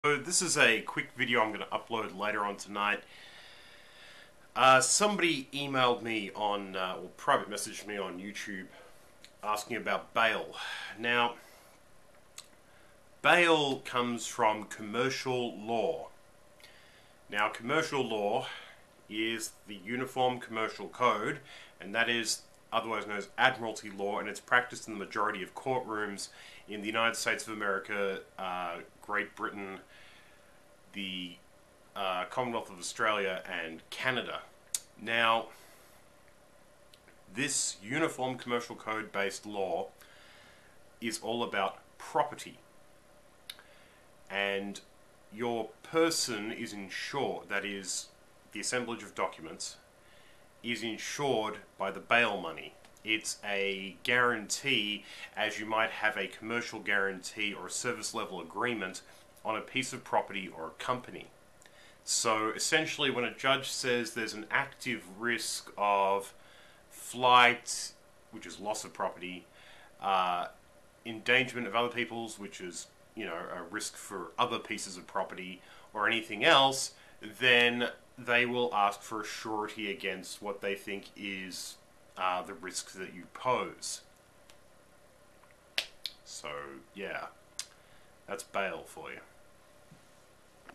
My understanding of bail money for HannahM666.
Basically bail money is insurance against property damage.

Sorry if I looked a bit scruffy, I was tired and overdue for a haircut.
A.G.